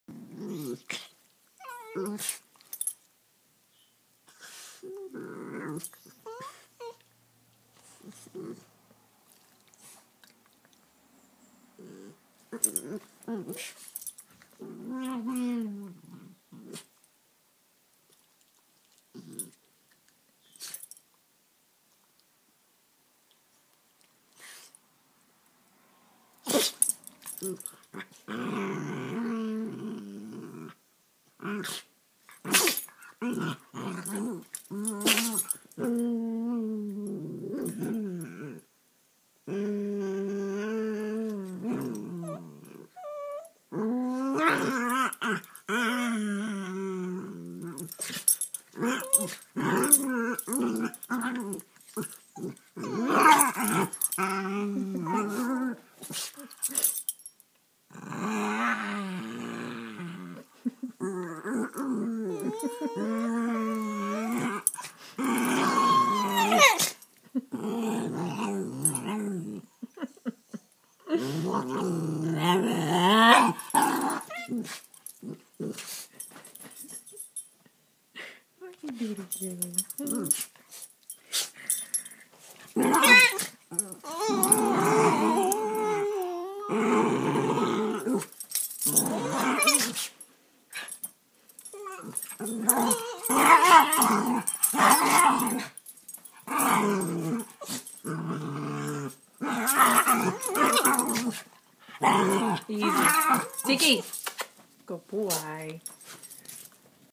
Mean Puppies Playing
Sammy, a rat terrier/Boston terrier mix, and Tiki, a beagle/Boston terrier mix playing like the big mean dogs that they are. Both will be turning two in May 2012. Both were rescues. No animals were hurt in the making of this film. :)